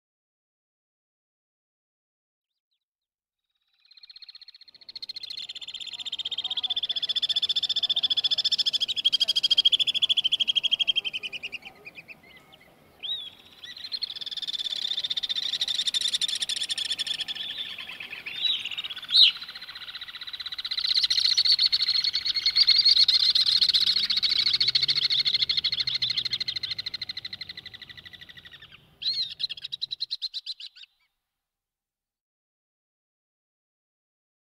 Malkar sorbeltza (Apus melba)
Dubrovnikeko (Kroazia) kaleetan hegan, 2018ko abuztuan.